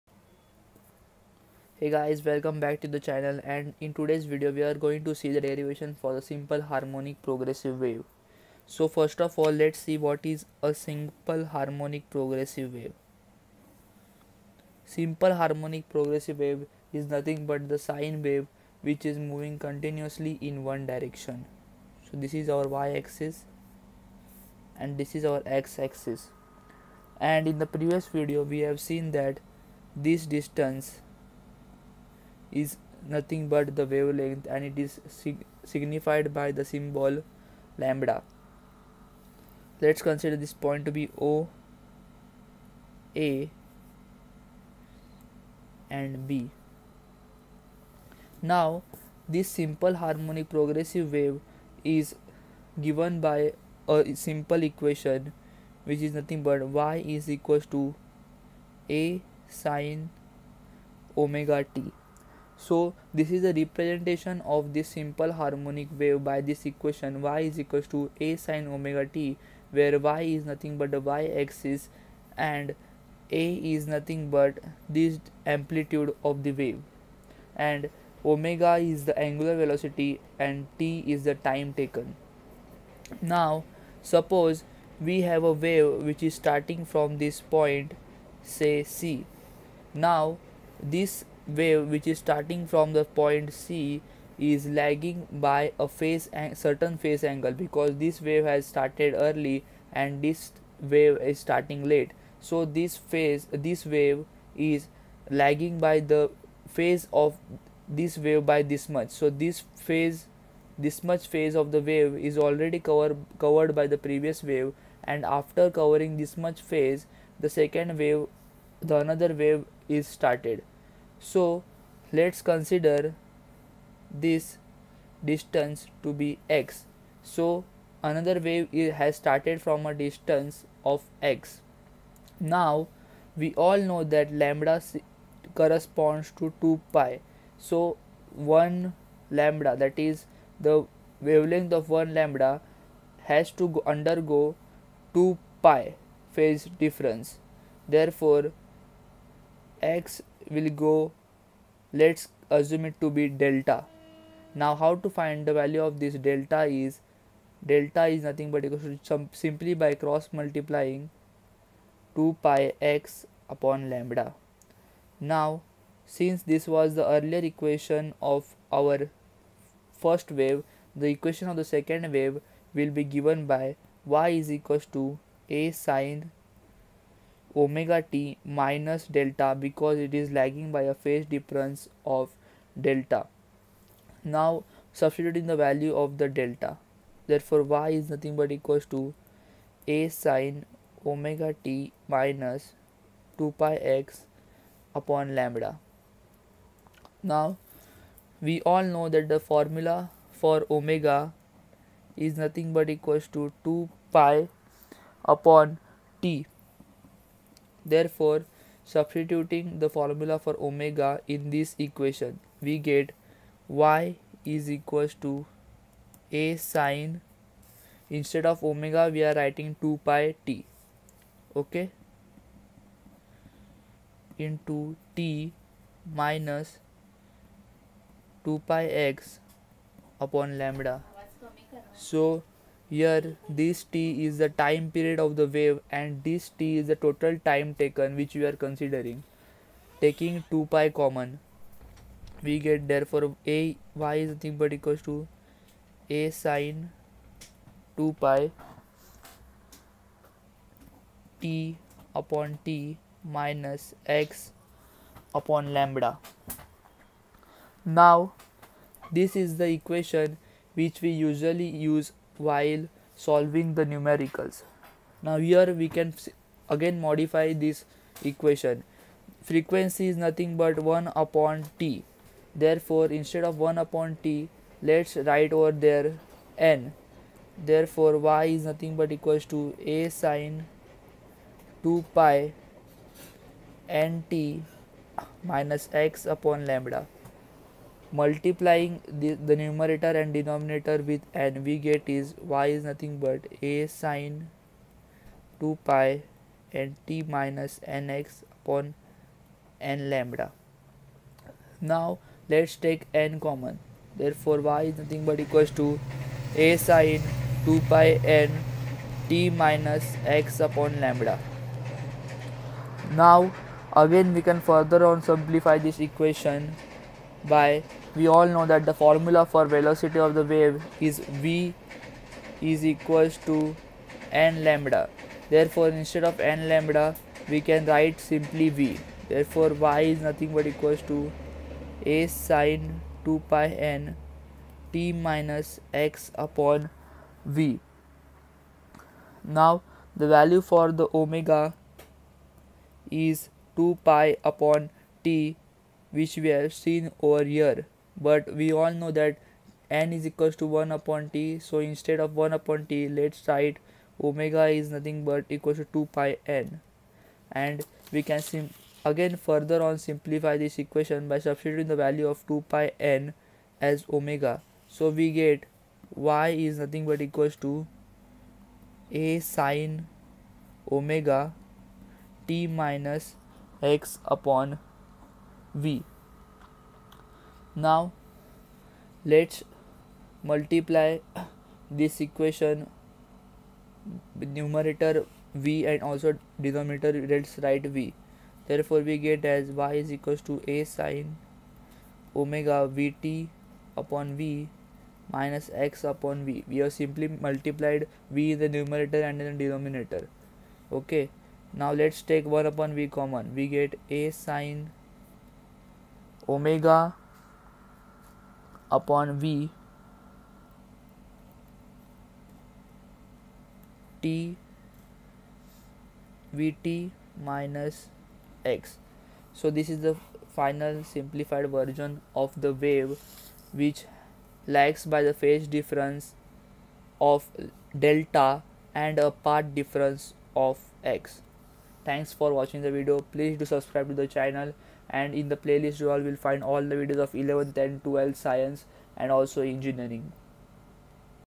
Simple Harmonic Progressive Wave / Derivation of simple harmonic progressive wave.
In this video the concept of simple harmonic progressive wave is explained along with the very easy derivation.

A simple harmonic progressive wave is a wave which continuously advances in a given direction without change of form and the particles of the medium perform simple harmonic motion about their mean position with the same amplitude and period, when the waves pass over them.

Characteristics of a Simple Harmonic Progressive Wave:
All particles vibrate with the same amplitude. All particles vibrate with the same frequency. The propagation of the simple harmonic progressive wave is a doubly periodic phenomenon, periodic in time and periodic in space. Energy is transmitted through the medium.